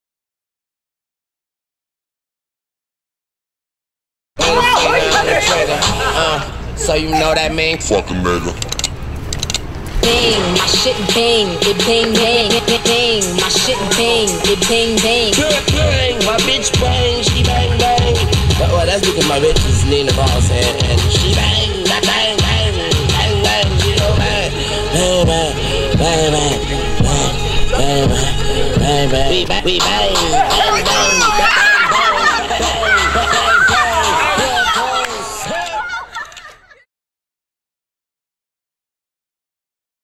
NCEconverse2
New Creative Entertainment, young urbane youth dancing in the street.